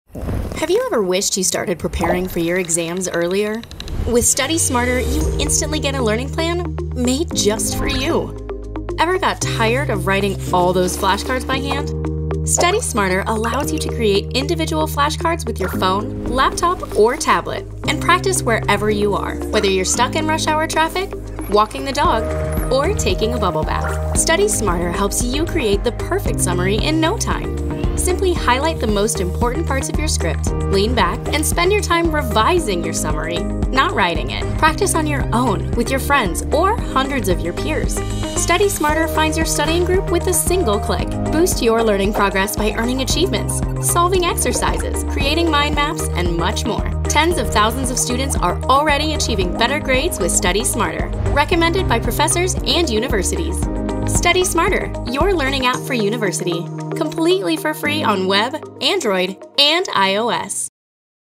StudySmarter - The all-in-one study app
StudySmarter is the first intelligent learning app to digitize the entire learning process of all students. Create and share flashcards, summaries, study plans, mind-maps and much more - faster and more efficient than ever.